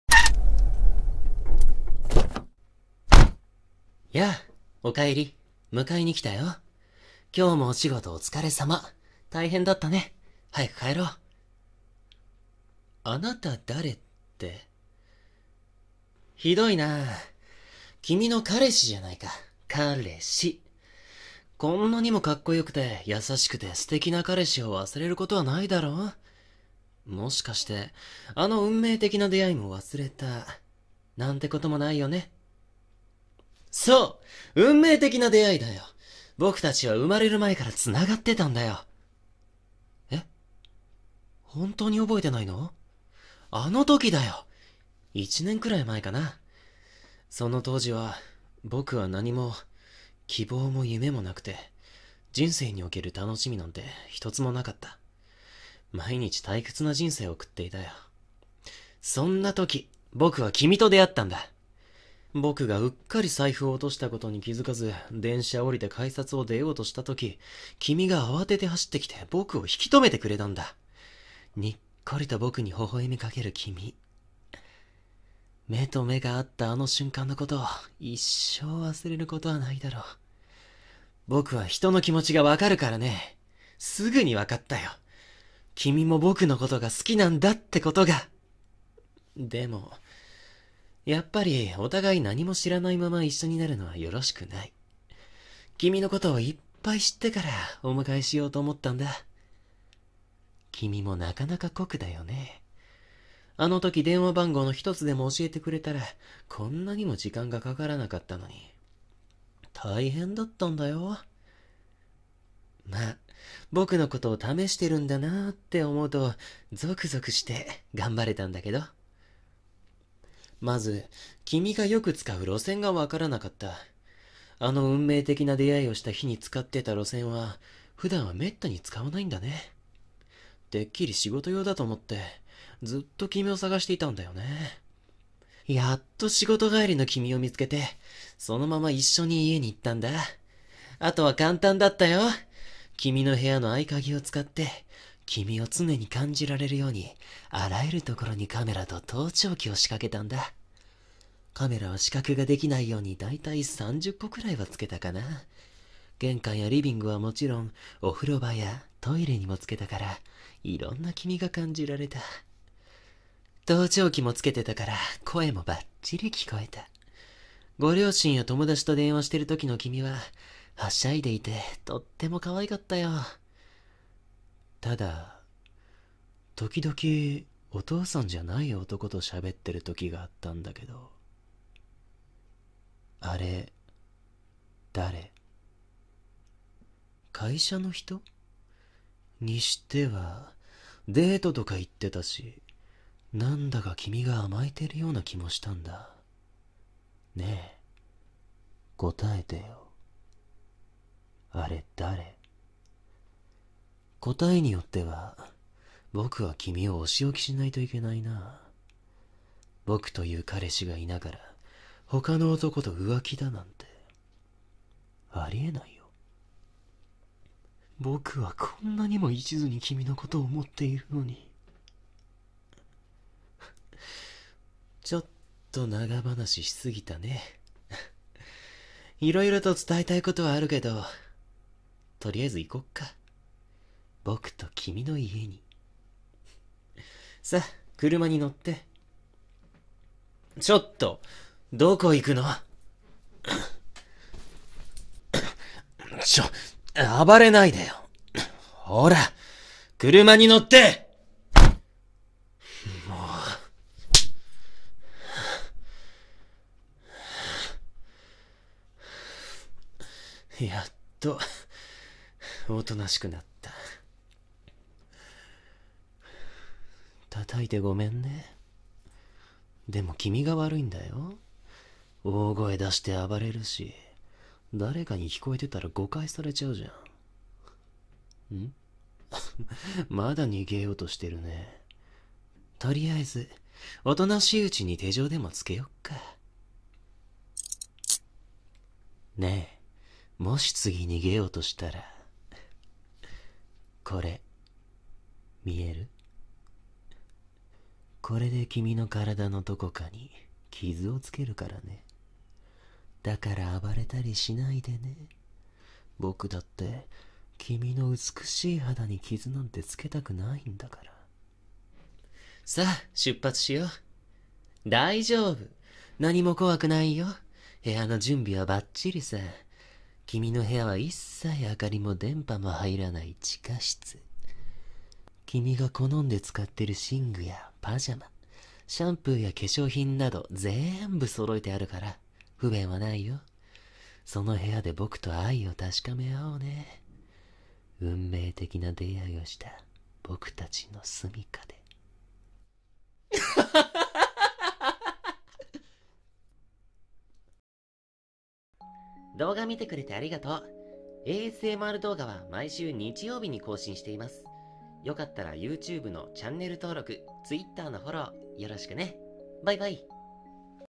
【女性向けASMR】ヤンデレのストーカーに誘拐される【バイノーラル,シチュエーションボイス,監禁】
今回の女性向けASMRはヤンデレ男に誘拐されるシチュエーションボイスです

台本：レテ 様
『迎えにきたよ、僕の運命の人』

※ASMR動画は毎週日曜日に投稿してるよ